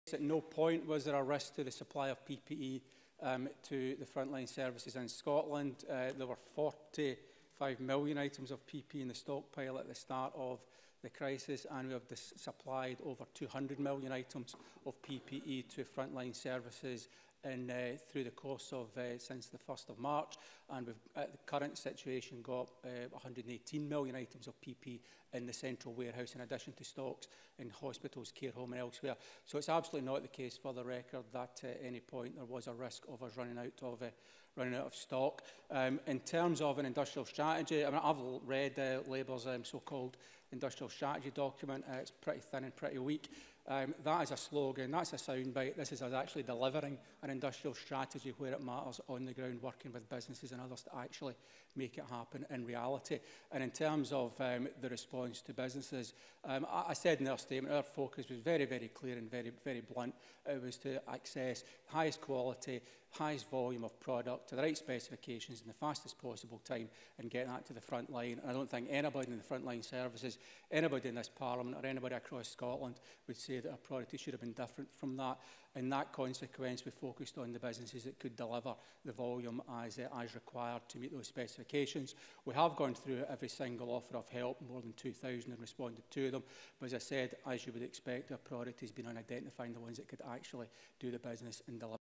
Ivan McKee MSP - Scotland's PPE Stock, Supply & Procurement, 27 May 2020
Ivan McKee MSP, Scottish Government Minister for Trade, Investment & Innovation.
Shortages of PPE for Scotland's PRIVATE & third sector nursing homes & Care Agencies was created by Public Health England instructing suppliers to only supply those registered with CQC, (Care Quality Commission), England; They are now, & have been for some time, able to supplement their PPE shortages from Scotland's Public Health Stock.